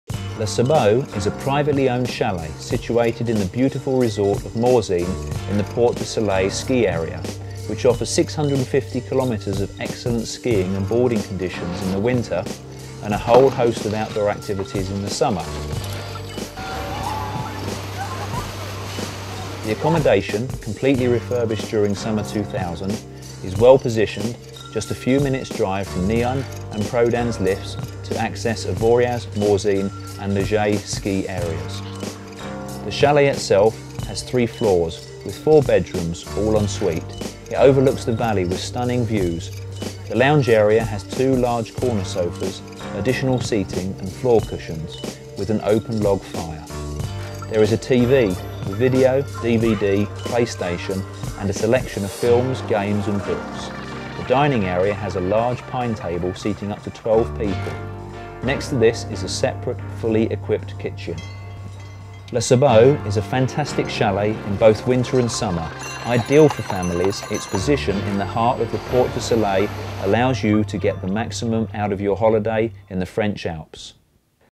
Chalet Le Sabot, Morzine
Lovely Chalet located in Morzine, France available for rentals during the Ski season and summer too.